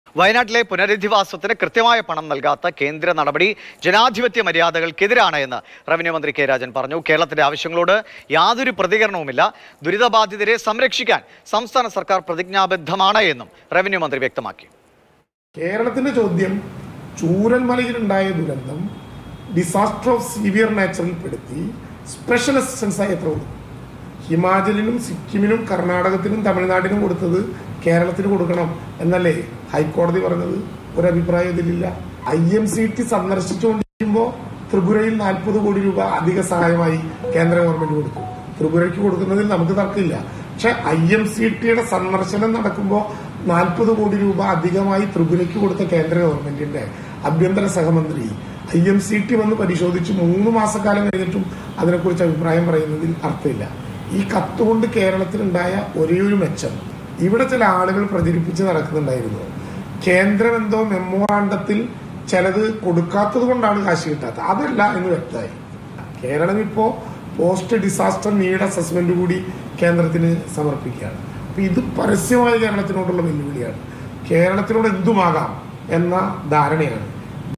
കേന്ദ്രത്തിന്‍റെത് പരസ്യ വെല്ലുവിളി; കേരളത്തിനോട് എന്തും ആകാം എന്ന ധാരണ- കെ രാജൻ
വയനാട് പുനരധിവാസത്തിന് കൃത്യമായ പണം നൽകാത്ത കേന്ദ്ര നടപടി ജനാധിപത്യ മര്യാദകൾക്ക് എതിര്. കേരളത്തിനോട് എന്തും ആകാം എന്ന ധാരണയാണെന്ന് മന്ത്രി കെ രാജൻ
മാറുന്ന ലോകത്ത് കൂടുതൽ മാറ്റങ്ങളോടെ. പുതിയ വേഗത്തിൽ. പുതിയ ലുക്കിൽ.
മാതൃഭൂമി ന്യൂസ്.

Happy viewing!